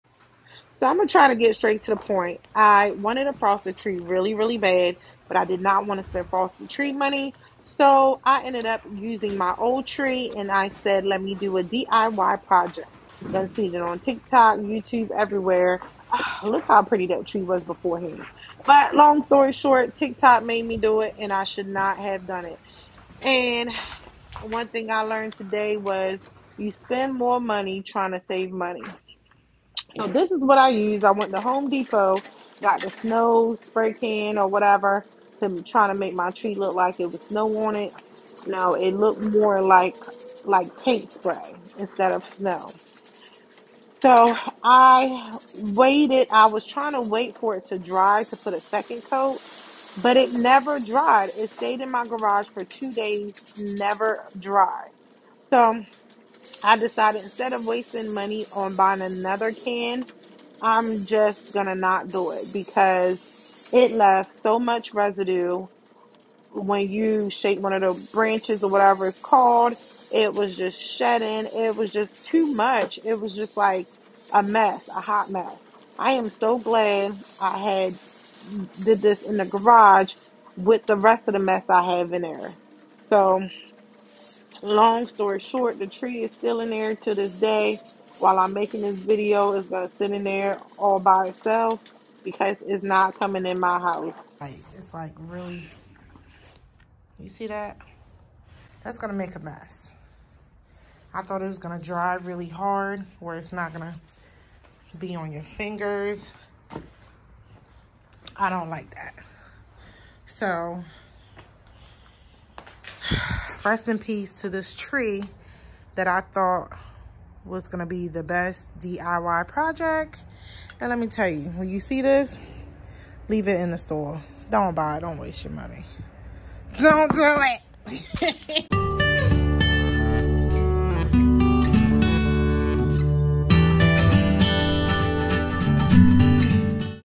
What They DON'T Tell you about FAKE SNOW SPRAY !
So I decided I wanted a different look for my tree this year, and wanted the tree to look like frosted snow. Bought 1 can of fake snow from Home Depot .